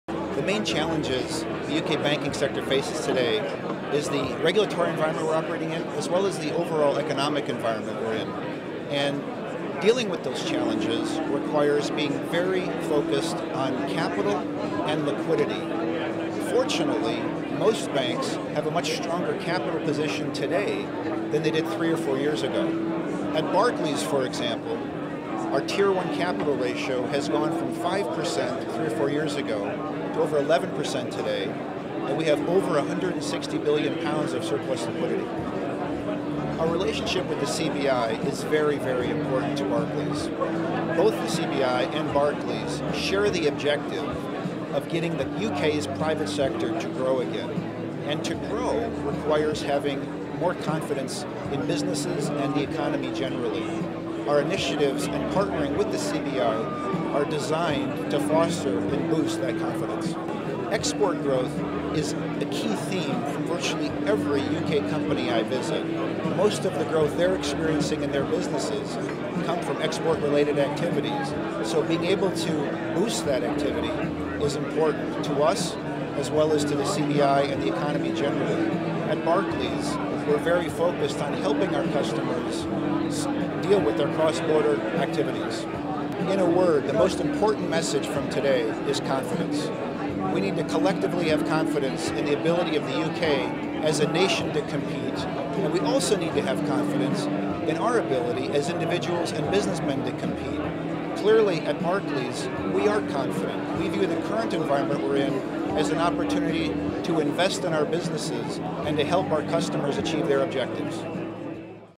John Winter, Barclays Corporate CEO, at CBI Annual Conference
Barclays Corporate CEO John Winter says that most banks have a much stronger capital position today than was the case  three or four years ago